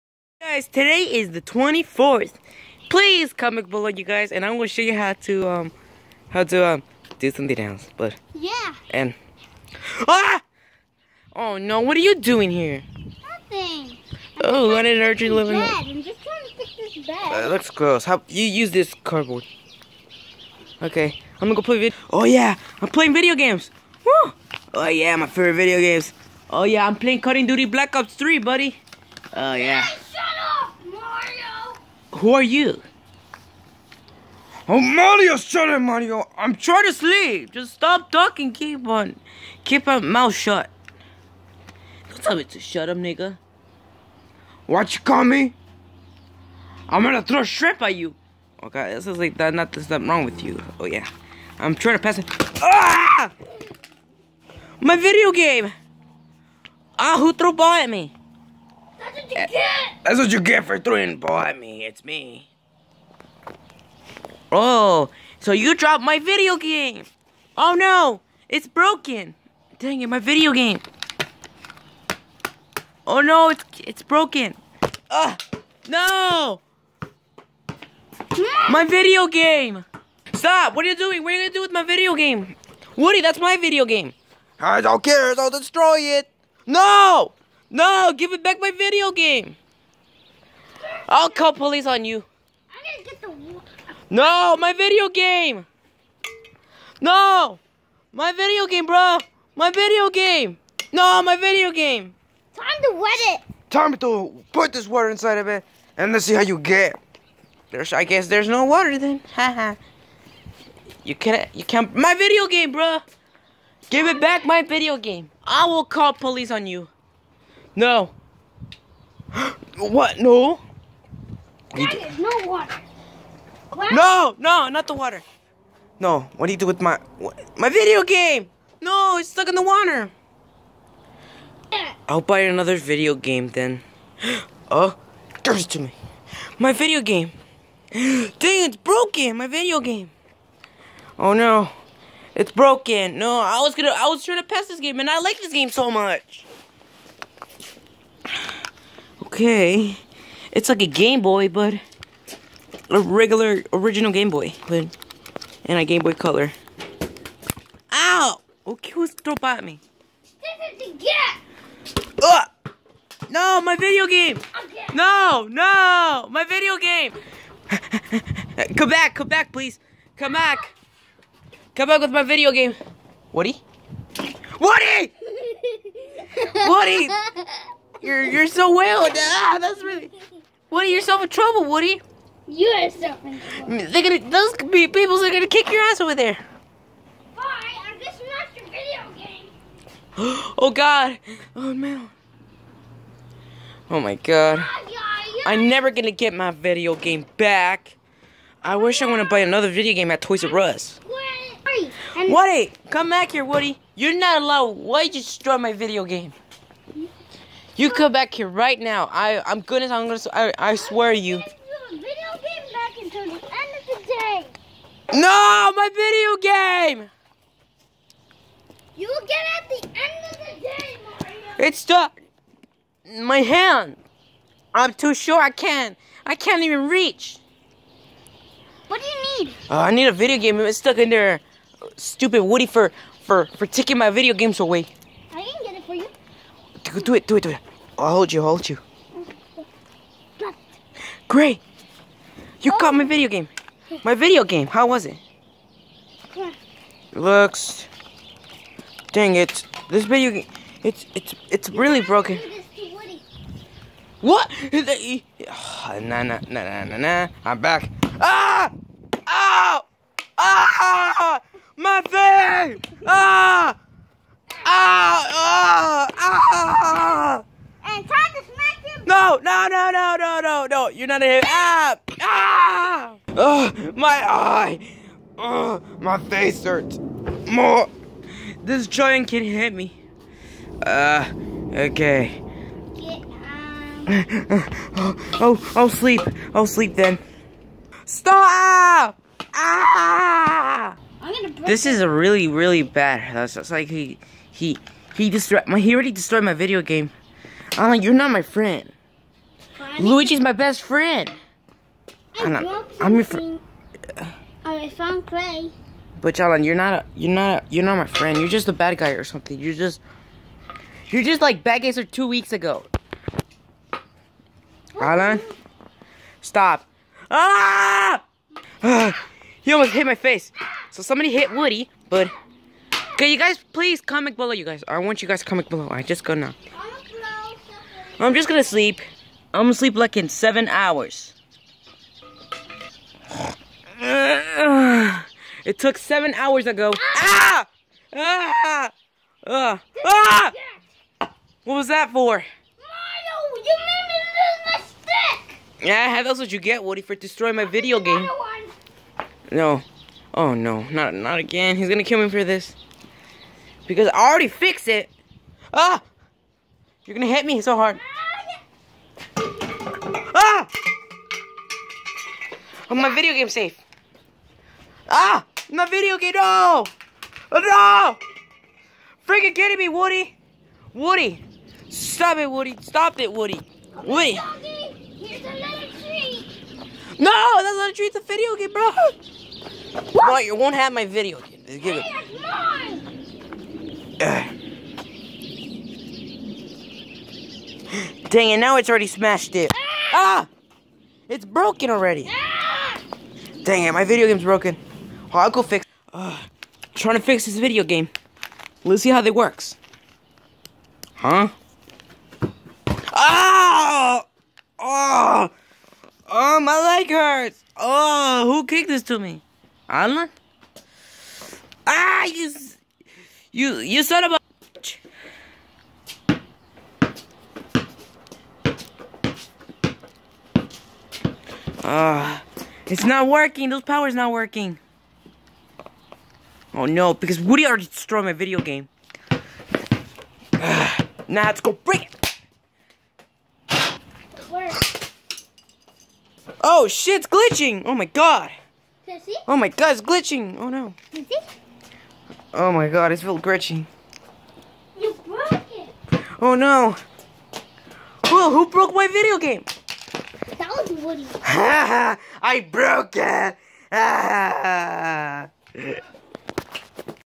part 7 woody destroy Mario his game
went Mario playing his video games and woody was smashing Mario game woody freak out he in trouble